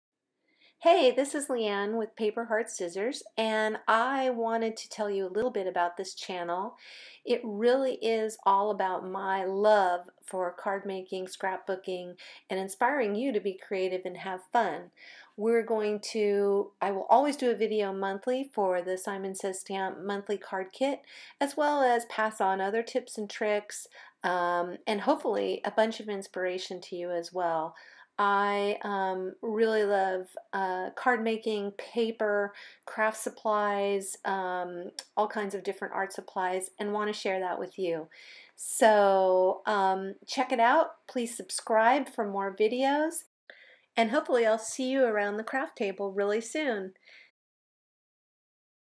About Paper Heart Scissors Handmade Cards and Papercrafts
Sharing my love for paper, art supplis and handmade cards and stationary, stamping and scrapbooking.